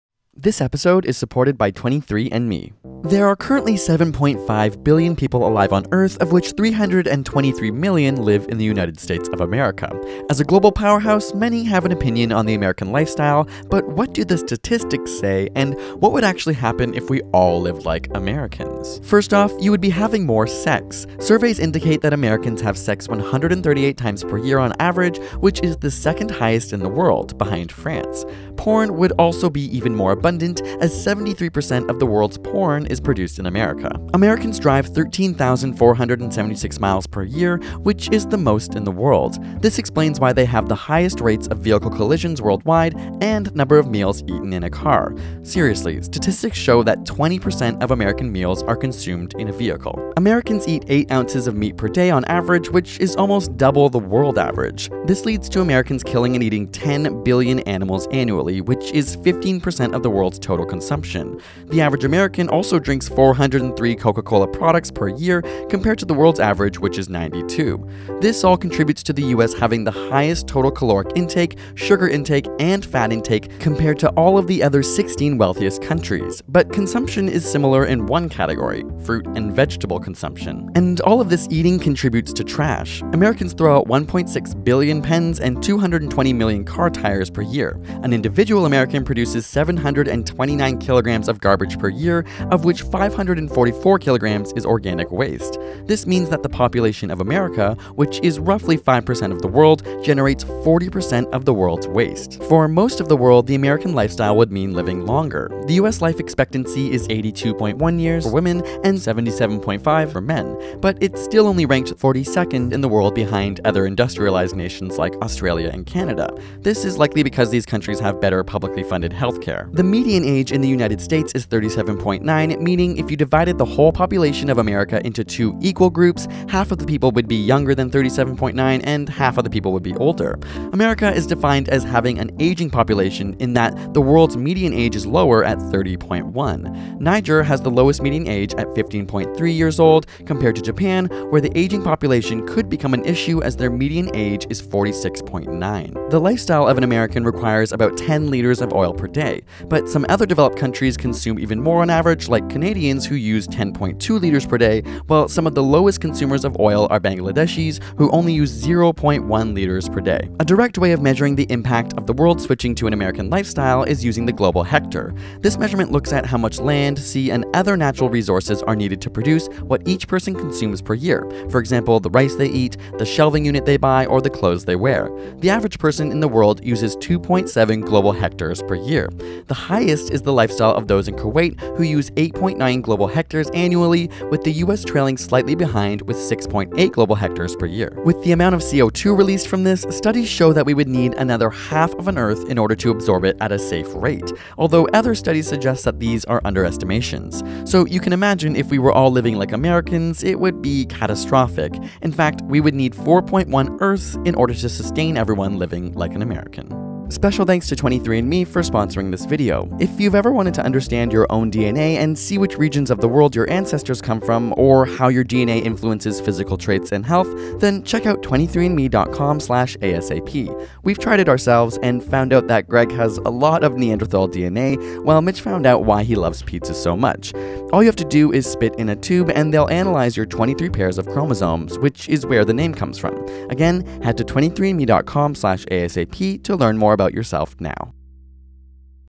What If Everyone Lived Like Americans?
What if the whole world was like America?

Created by: Mitchell Moffit and Gregory Brown
Written by: Gregory Brown
Illustrated by: Max Simmons
Edited by: Sel Ghebrehiwot & Mitchell Moffit

SNAPCHAT US 'whalewatchmeplz' and 'pixelmitch'

ASAPSCIENCE INC.